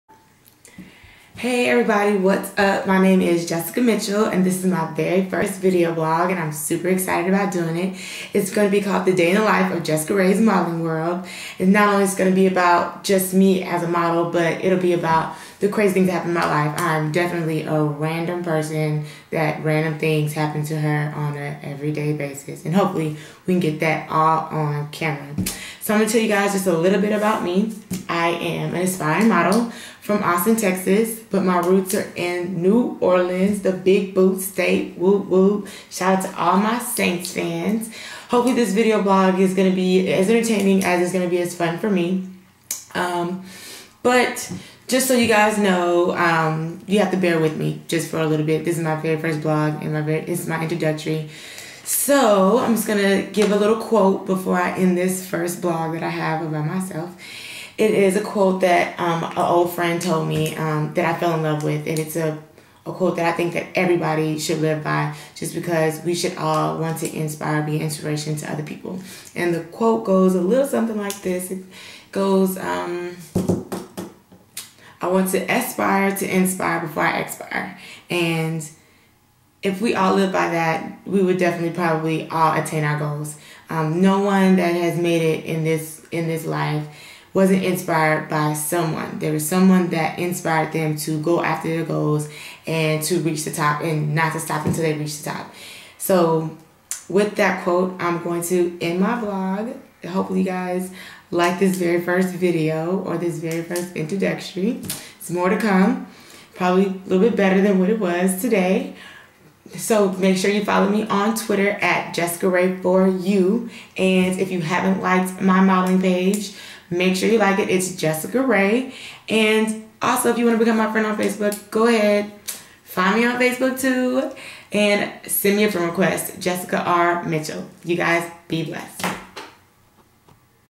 A Day In The Life Of Jessicarea's Modeling World.MPG
This is My Very 1st Video Blog The Intro To the Day In the Life Of JessicaRea's Modeling World!!